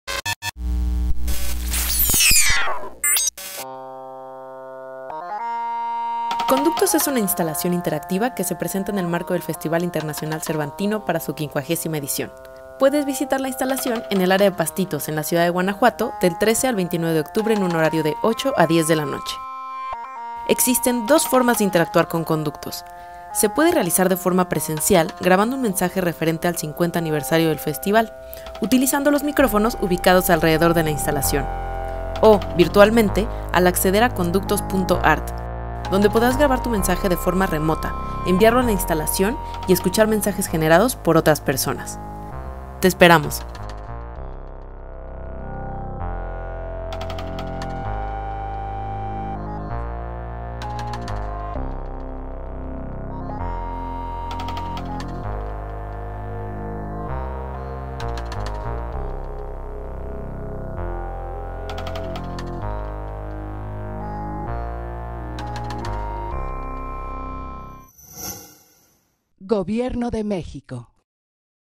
Conductos
Pieza sonora y lumínica de Humedales Digitales

Conductos es una pieza lumínica y sonora que forma parte de Humedales digitales, un programa híbrido desarrollado por el Centro de Cultura Digital, México Creativo y el Centro Cultural de España en México a presentarse en el 50º Festival Internacional Cervantino. La pieza fue desarrollada en La Colmena: Centro de Tecnologías Creativas "Grace Quintanilla" con la colaboración de estudiantes del Colegio de Estudios Científicos y Tecnológicos de Tlaxcala.

Uno de los elementos más representativos de la ciudad de Guanajuato es su red de túneles que, desde tiempos de la colonia, conectan plazas con teatros, jardines o palacios. Una verdadera infraestructura de comunicación entre personas y espacios que nos invita a reparar en los conductos que hacen posible estos vínculos. Igual que las carreteras y la infraestructura establecen nexos entre distintas regiones, la tecnología construye redes de comunicación entre personas apartadas por la distancia.

Conductos es un homenaje a esa capacidad que ha tenido el FIC para ser medio, ser vínculo, ser infraestructura, en los ámbitos de la cultura. La pieza permite la comunicación entre puntos remotos conectados por túneles de datos que recuerdan las redes de comunicación y colaboración que el festival ha tendido más allá de las fronteras de Guanajuato y México.

Está intervención será el nodo central de una red de experiencias interconectadas en un ejercicio de intercambio de mensajes sonoros, con la finalidad de compartir vivencias, deseos y recuerdos sobre el FIC.

La instalación cuenta con dos vectores que vinculan la experiencia digital con el entorno de Pastitos.
Vector digital
La participación colectiva de la pieza se dará a través de una aplicación web en la que les usiaries podrán grabar un audio que se desplegará en el vector físico instalado en Pastitos. Estas interacciones afectarán las dinámicas sonoras y lumínicas de la pieza en tiempo real.

Nota: el link a la aplicación estará disponible próximamente.

Vector físico
La experiencia inmersiva de la pieza será en la zona de Pastitos, donde habrá una pantalla cúbica de leds de cinco metros de alto que proyectará animaciones tridimensionales. Además de los mensajes de la aplicación, habrá un equipo de micrófonos disponibles para grabar audios que afectarán en las imágenes desplegadas en la matriz en tiempo real.